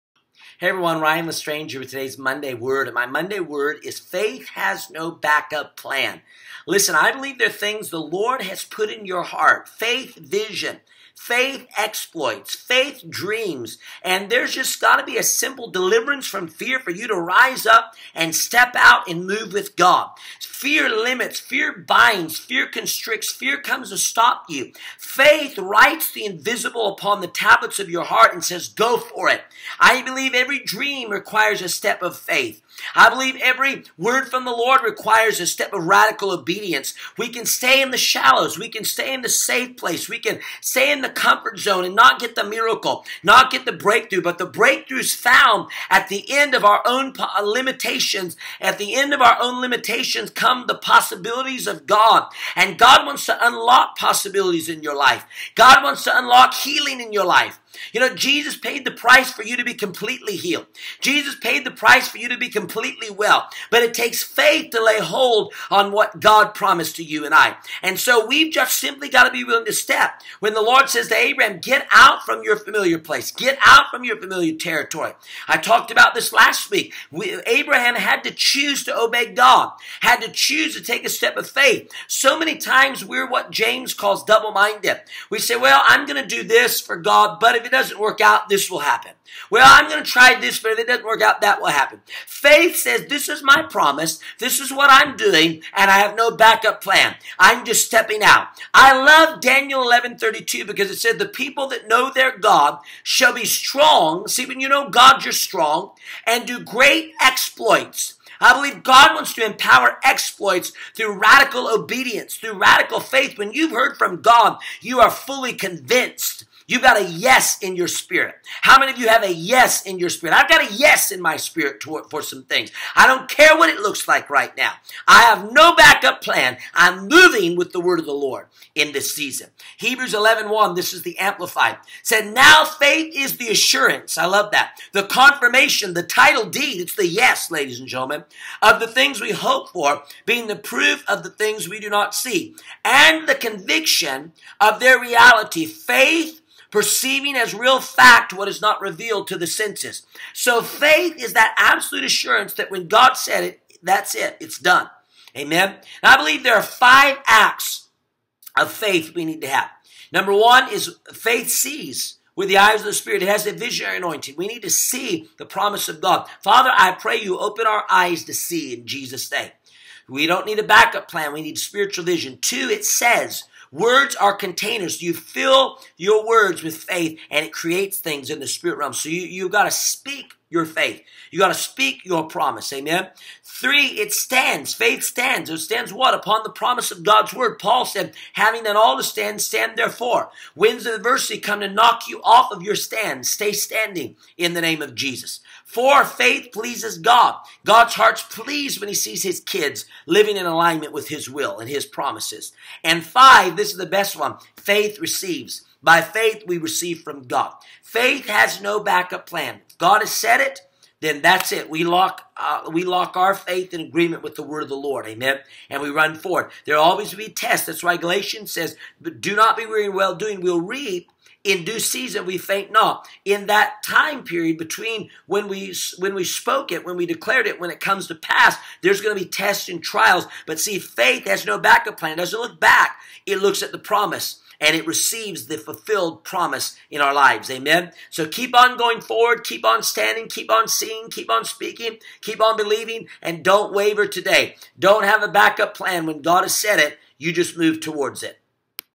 MONDAY WORD-Faith has no back up plan!
In this video Ryan talks about 5 dimensions of faith & how to get the breakthrough!